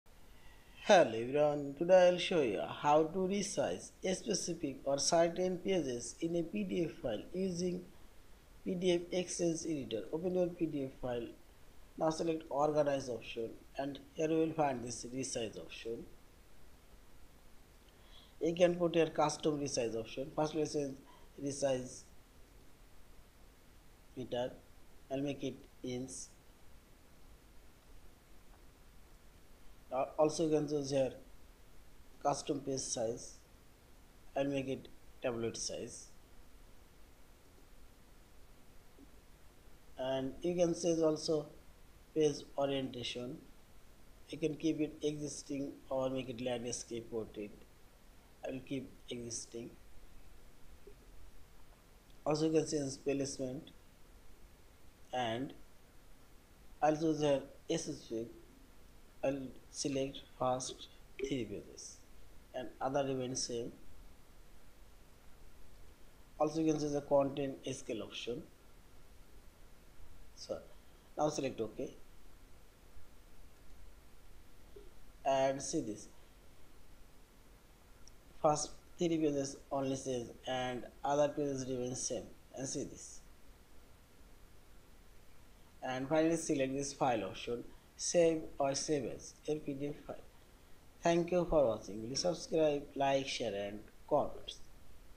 How to resize certain pages in PDF XChange Editor
Hello everyone, Today I will show you, How to resize certain pages in PDF XChange Editor.